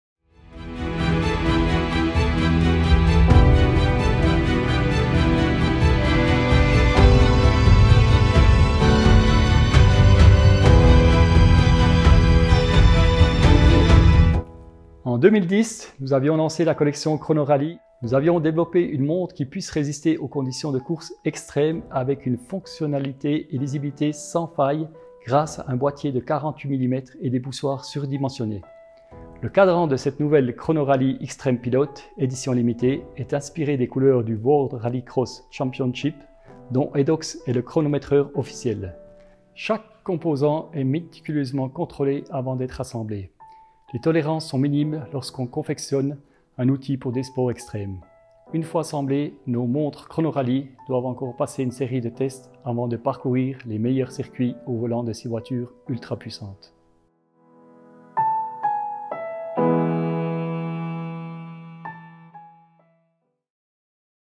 EDOX - CHRONORALLY X-TREME PILOT LIMITED EDITION - DOCUMENTARY
Discover all the technical aspects of the EDOX - Chronorally X-TREME Pilot